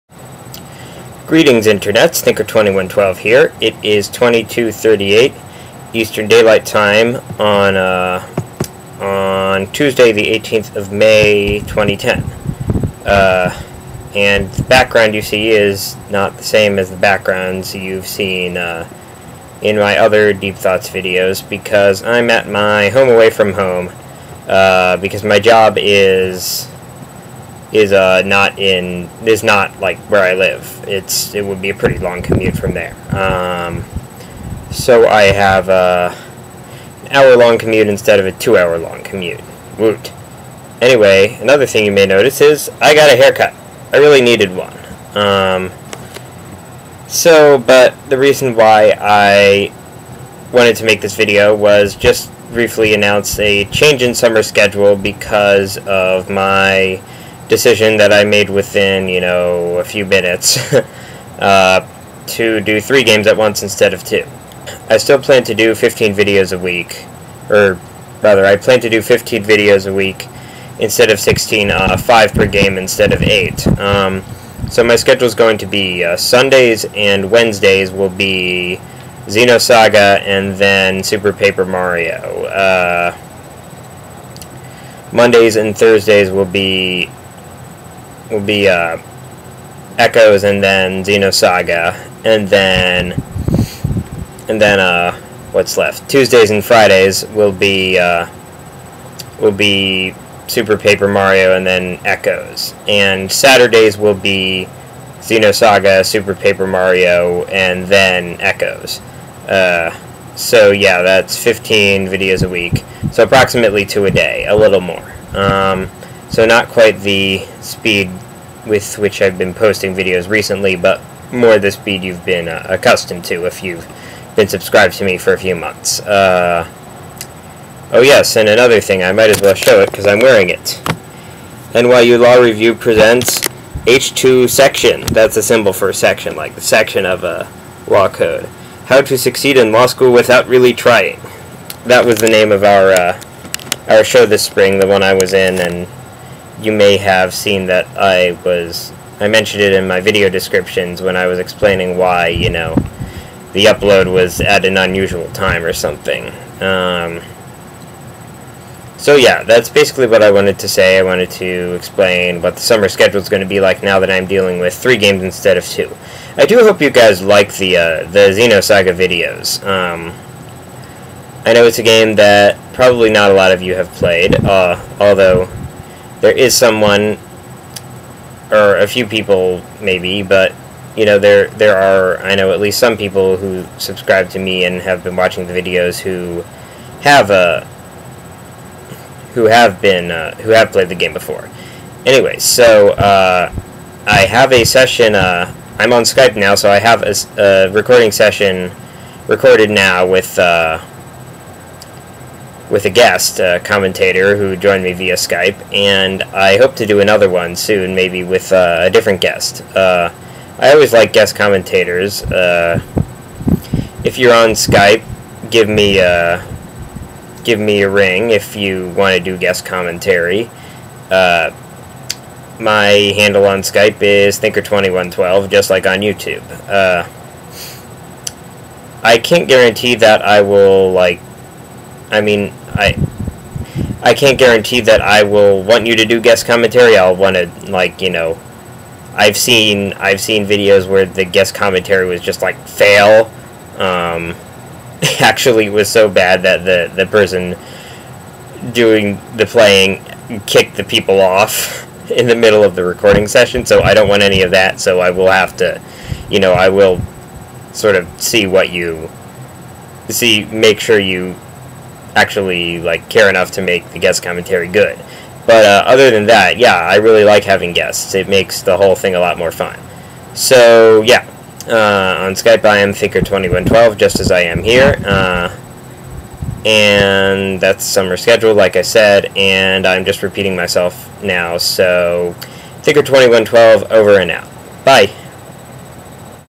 Thinker2112's Deep Thoughts, Episode 6: 18 May 2010 (Revised Summer Schedule and Skype)
I finally got a haircut!  Here's my revised summer schedule:
Sunday and Wednesday: Xenosaga, then Super Paper Mario
Monday and Thursday: Metroid Prime 2, then Xenosaga
Tuesday and Friday: Super Paper Mario, then Metroid Prime 2
Saturday: Xenosaga, then Super Paper Mario, then Metroid Prime 2

My Skype address is Thinker2112.  I hope to get some more guest commentators!